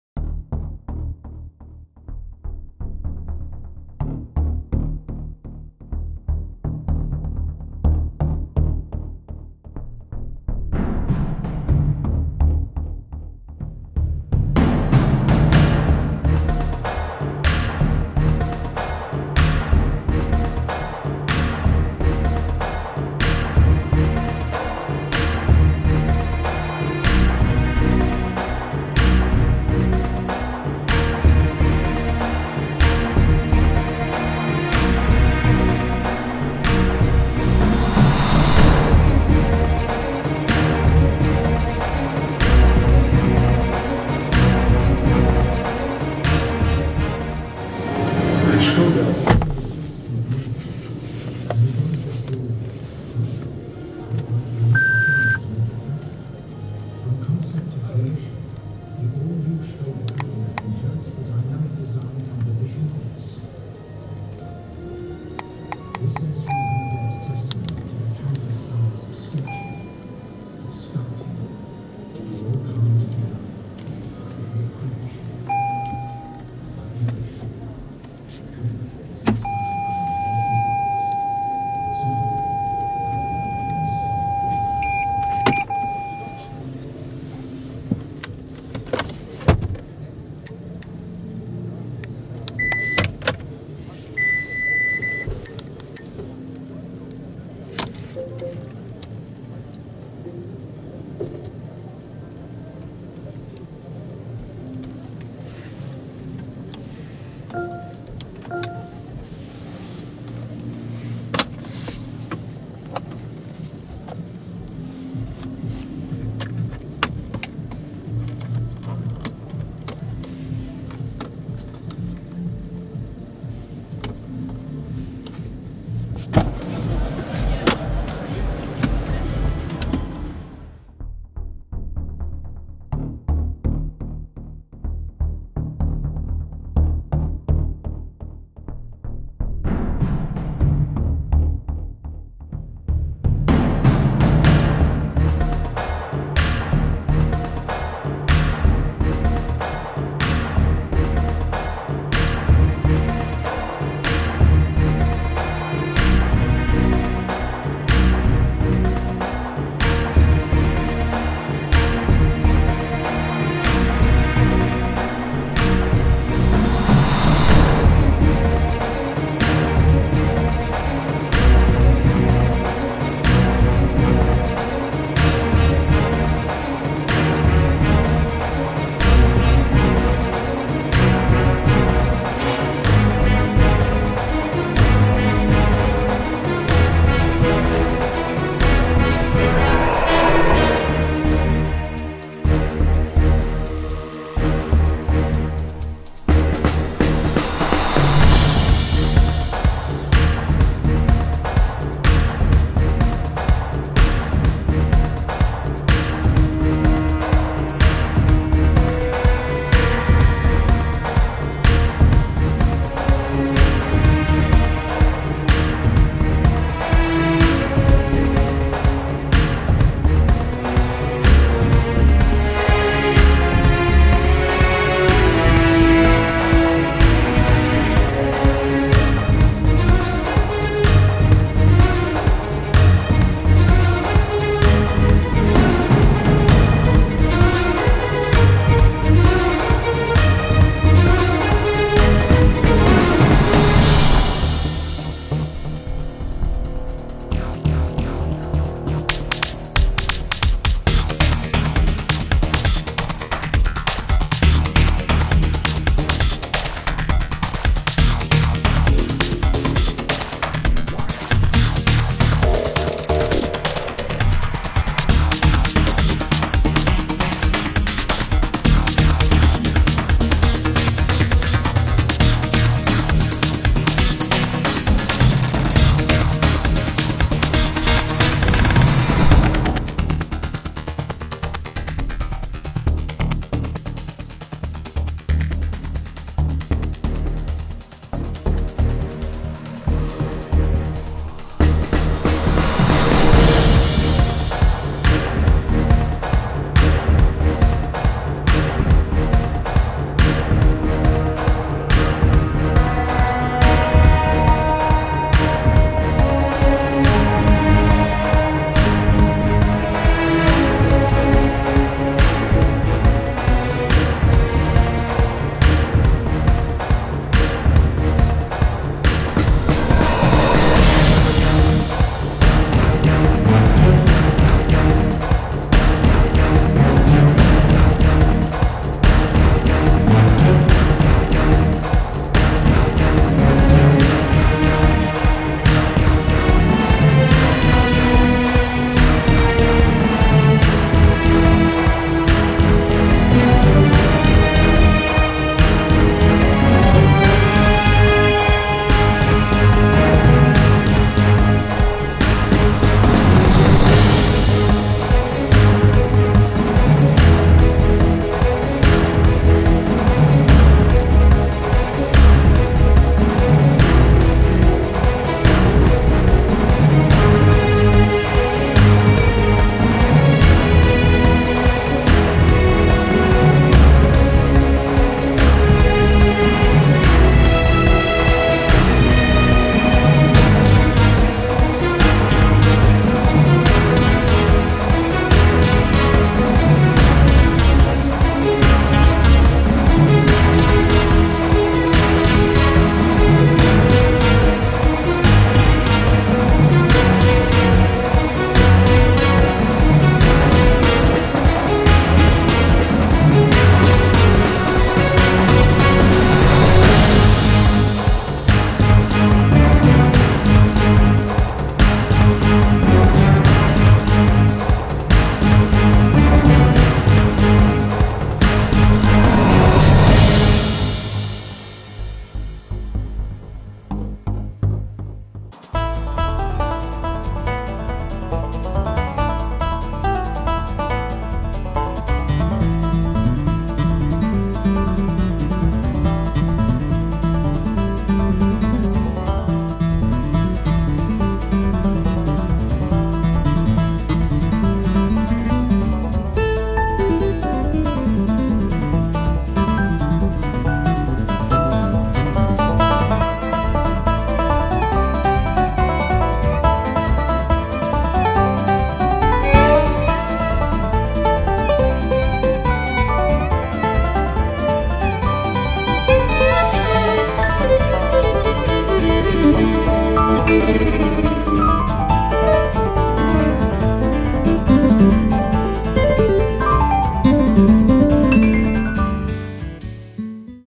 Lexus RX 450h F Sport (2017) Exterior and Interior in 3D
2017 Lexus RX 450h F Sport car seen from outside and inside in 3D.